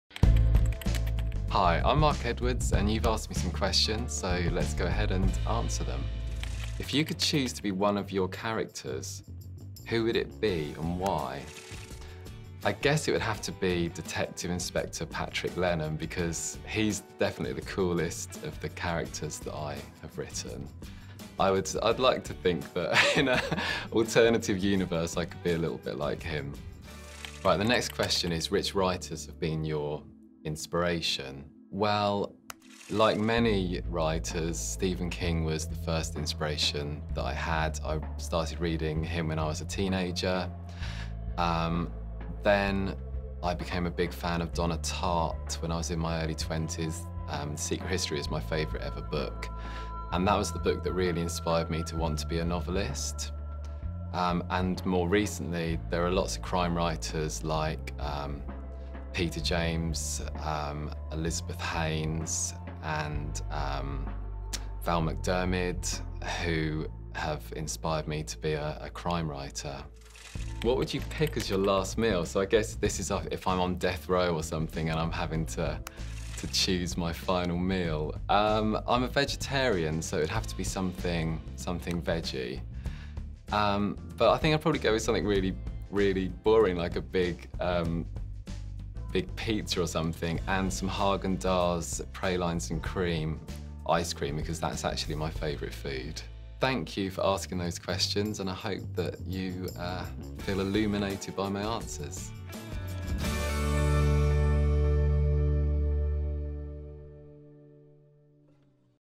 Fan Questions: Mark Edwards
“Like many writers, Stephen King was the first inspiration that I had,” says Mark Edwards. “Then I became a big fan of Donna Tartt when I was in my early twenties.” Learn more about Mark’s favorite crime authors, the character that’s most like him, and what he would eat for his last meal. Be sure to check out Mark’s latest novel, The Devil’s Work, available now.